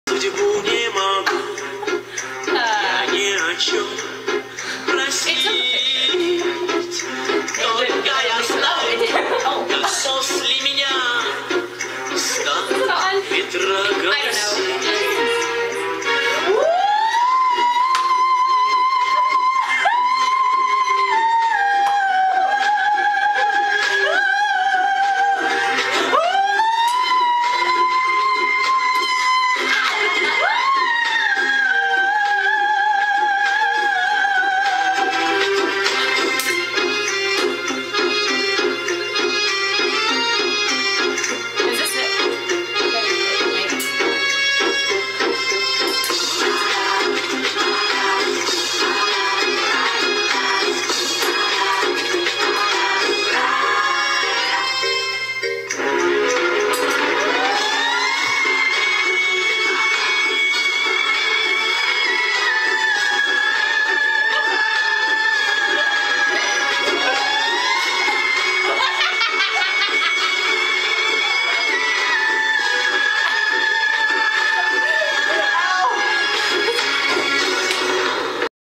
Better than Vitas
Four CRAZY college girls listened to Vitas on youtube and thought it'd be fun to do their own reenactment!